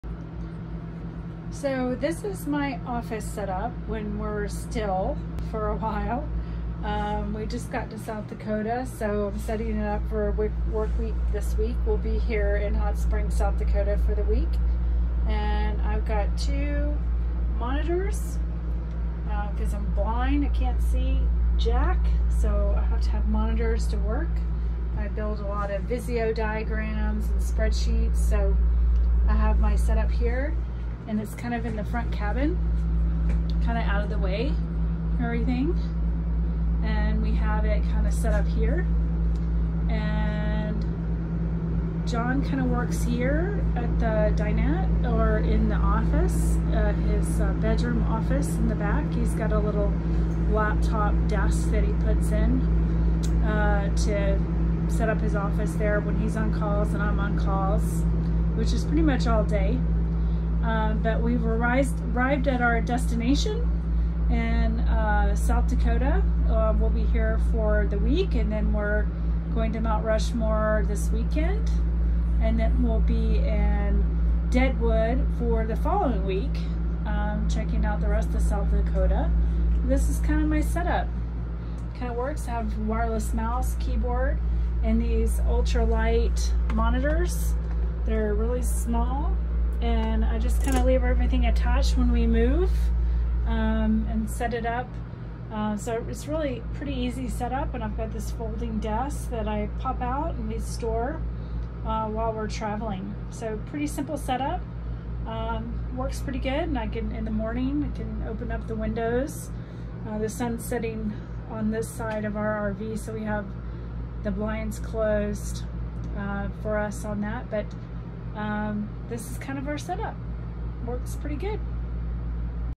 Home Office Set up in an RV
This is my home office set up in our Winnebago Forza 34T.